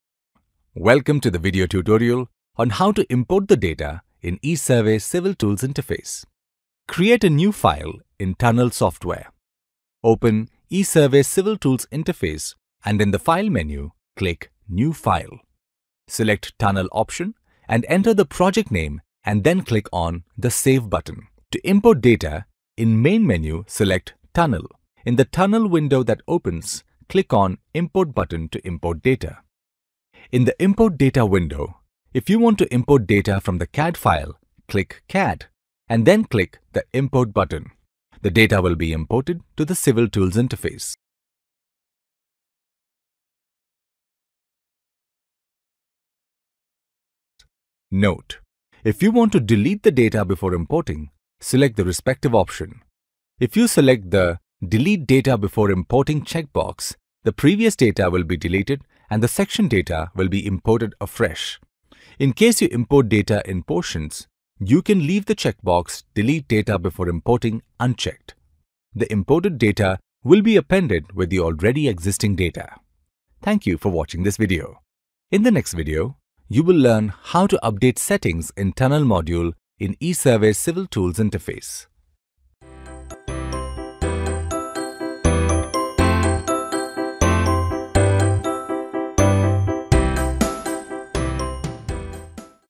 Prepare Tunnel Cross Sections by Importing CAD Data
Cross Sections of Tunnel can be prepared by importing Tunnel Data from the AutoCAD drawing files. After preparing the drawings you can take Area report between any two tunnel layers.

Hello Everyone! Welcome to the video tutorial on how to import the data in ESurvey Civil Tools Interface.

Create a New File in Tunnel Software.
Open the ESurvey Civil Tools interface, and in the File menu, click New File.

Select the Tunnel option and enter the Project Name, and then click on the Save button.
To Import Data, In the Main Menu, Select Tunnel.
In the Tunnel Window that opens, click on the Import button to Import Data.

In the Import Data window, if you want to import data from the CAD file, click CAD and then click the Import button. The data will be imported to the civil tool's interface.

Note: If you want to delete the data before importing, select the respective option
If you select the ‘Delete Data before Importing’ checkbox, the previous data will be deleted and the section data will be imported afresh.

In case you import data in portions, you can leave the checkbox Delete Data before Importing unchecked. The imported data will be appended with the already existing data.

In this video tutorial, you have learned how to import the data in ESurvey Civil Tools Interface.
In the next video, you will learn how to update Settings in the Tunnel Module, in the ESurvey Civil tools Interface.